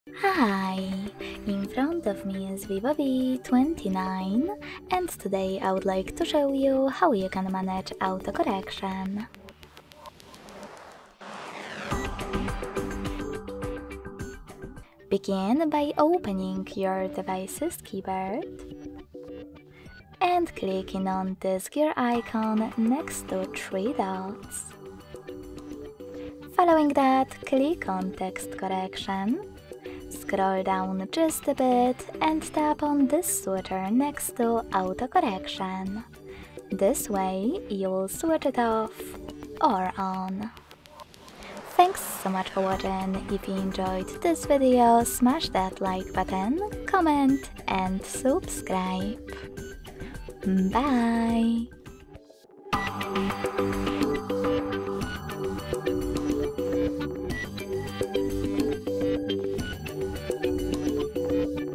How to Turn On/Off Auto Correction on VIVO V29?
Find out more:
This tutorial assists VIVO V29 users in controlling the auto-correction feature on their smartphones. Auto-correction automatically suggests or corrects typed words while using the device's keyboard, aiding in typing accuracy. The step-by-step guide provides clear instructions on how users can easily toggle the auto-correction setting on or off on the VIVO V29. By following this tutorial, users can personalize their typing experience according to their preferences.

Why manage auto-correction on the VIVO V29?
Where can users find the settings to toggle auto-correction on the VIVO V29?
Can users adjust the sensitivity or customize auto-correction preferences on the VIVO V29?
How does enabling or disabling auto-correction affect typing efficiency on the VIVO V29?
Will turning off auto-correction impact other keyboard functionalities on the VIVO V29?